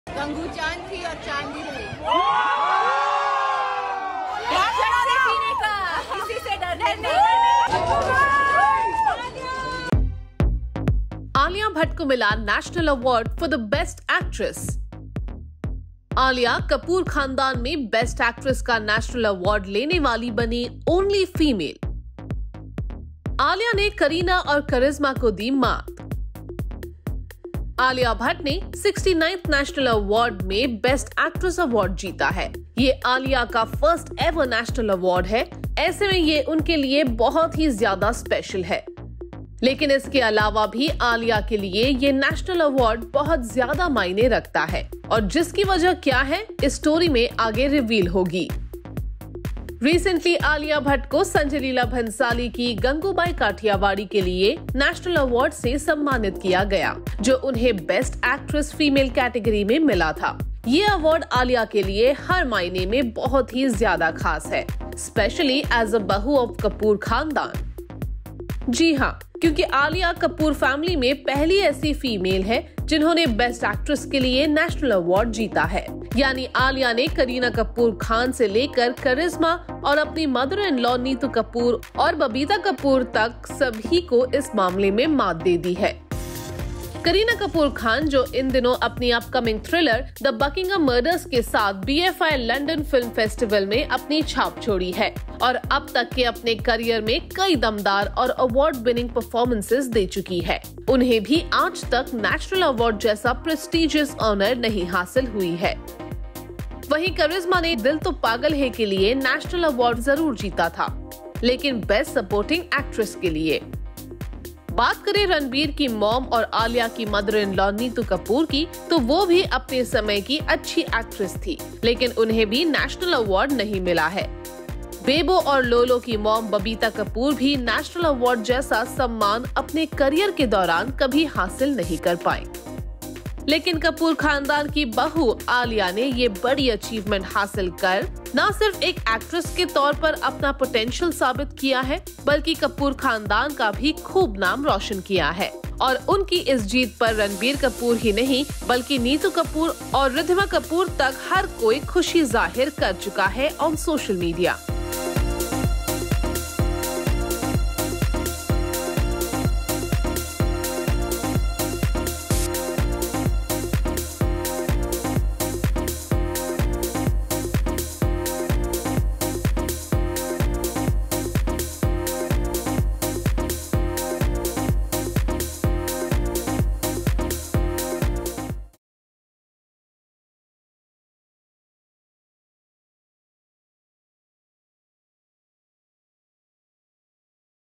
Alia Bhatt creates HISTORY in Kapoor family after winning 69th National Film Award
Bollywood News: Alia Bhatt came to New Delhi for the 69th National Film Awards. She won the 'Best Actress' prize for her amazing role in the movie 'Gangubai Kathiawadi.' She's the first Kapoor woman to achieve this honor. Watch the video to know more.